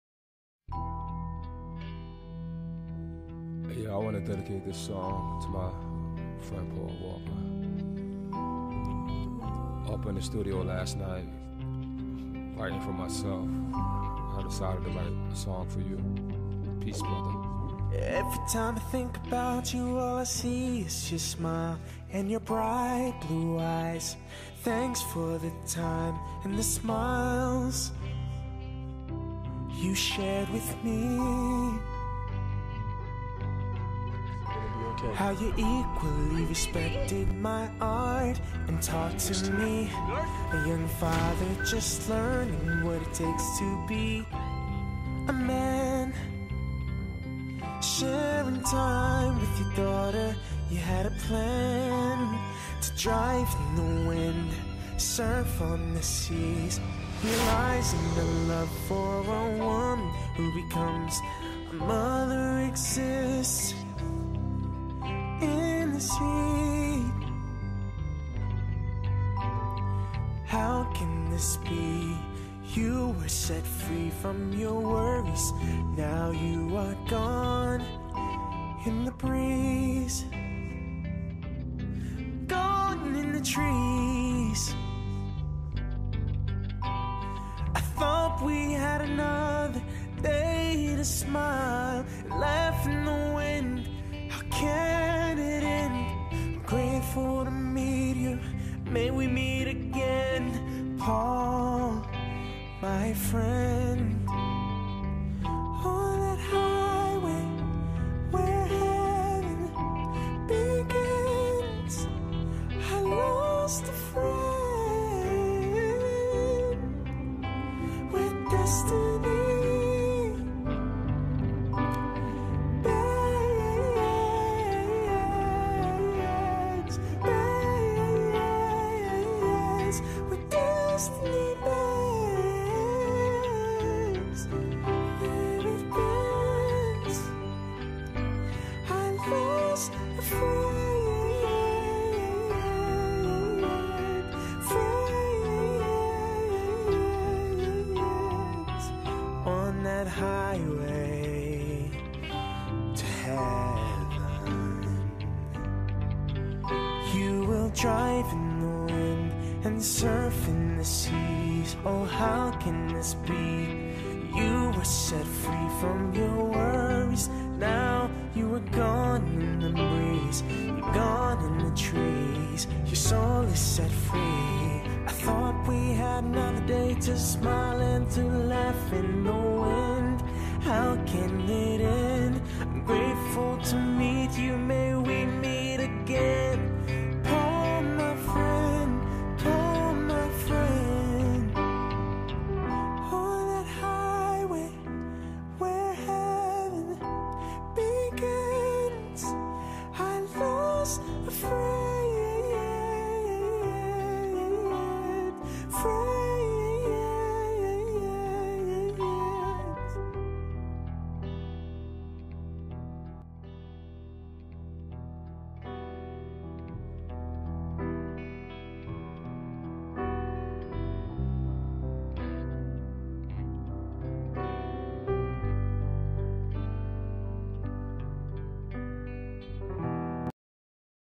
RIP Paul Walker
Paul Walker was one of my favorite actors for many reason, so I decide to make a tribute video in his honor and memory. RIP